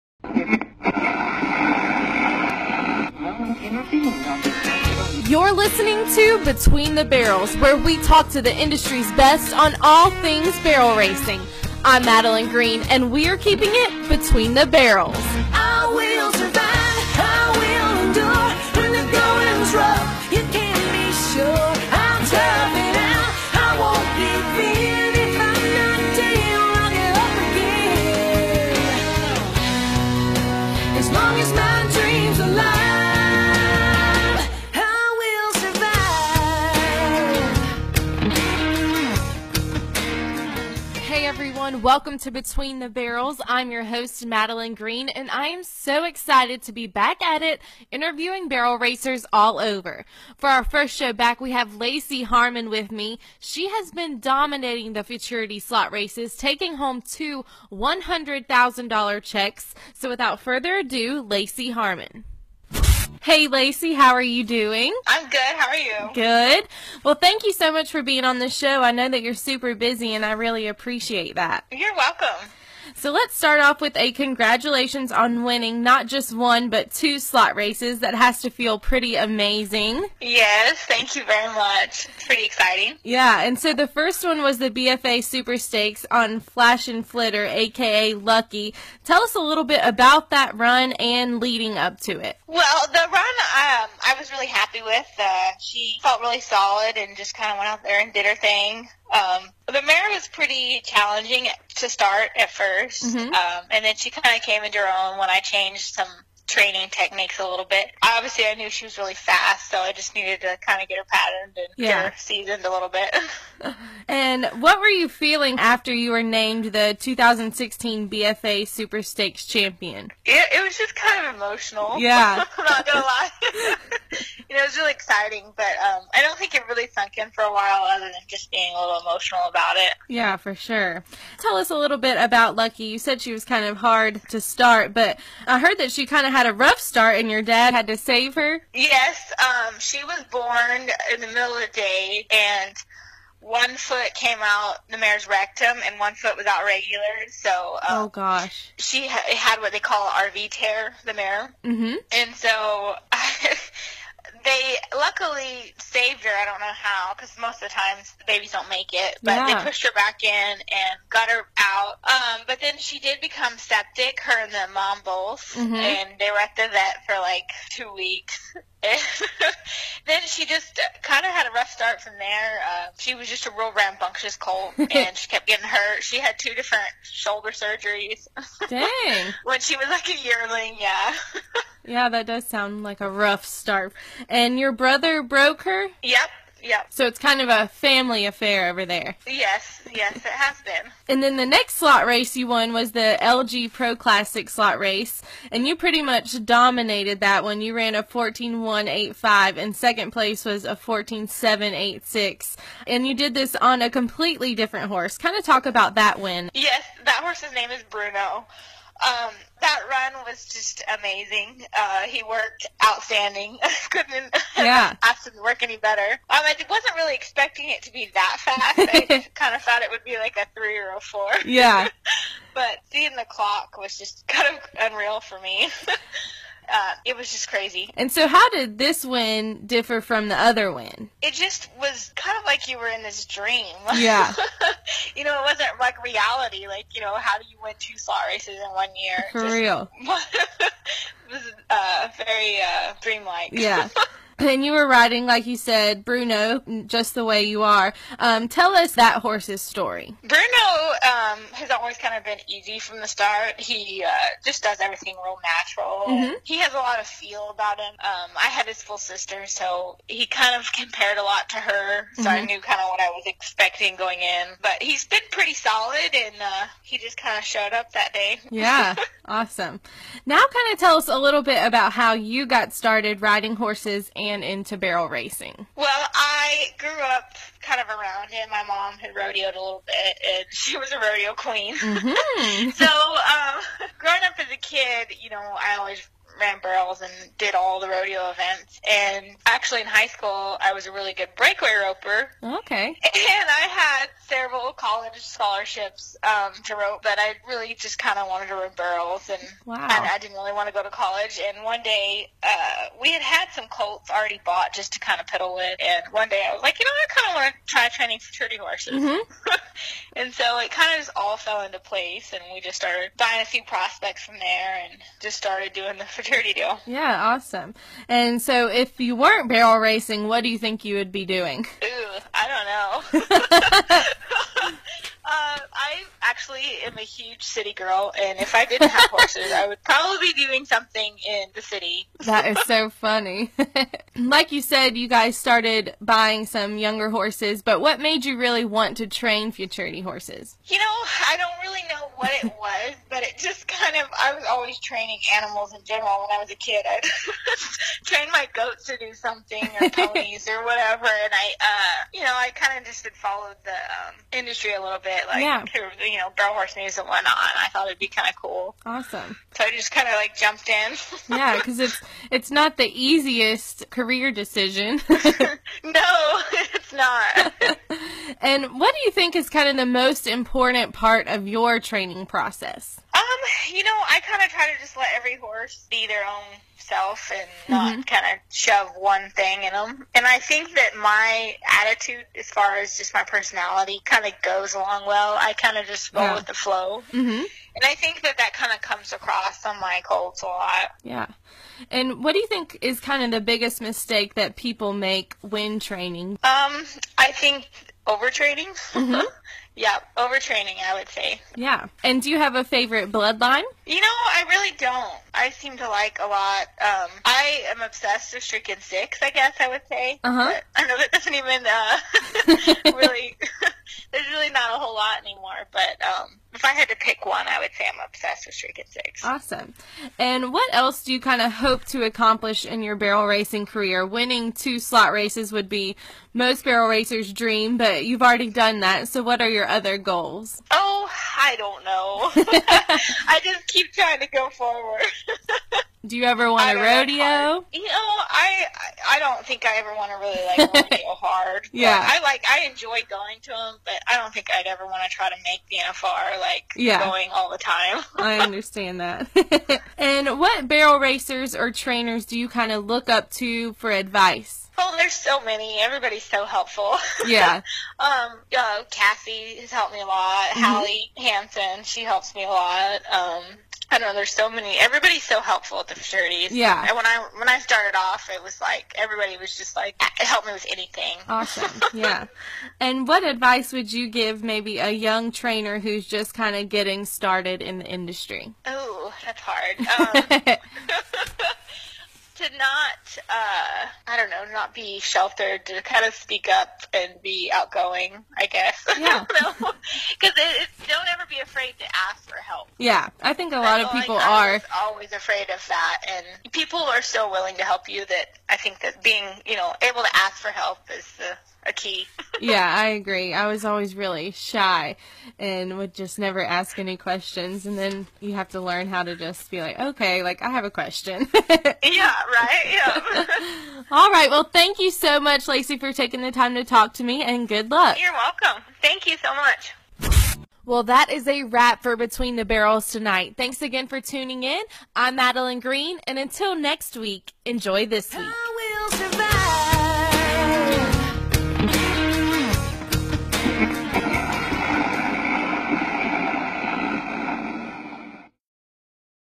Lacey Donegan on Between the Barrels
Lacey was on fire at the futurity slot races this year. Who wants to know what it feels like to win 2 of the biggest slot races in the industry.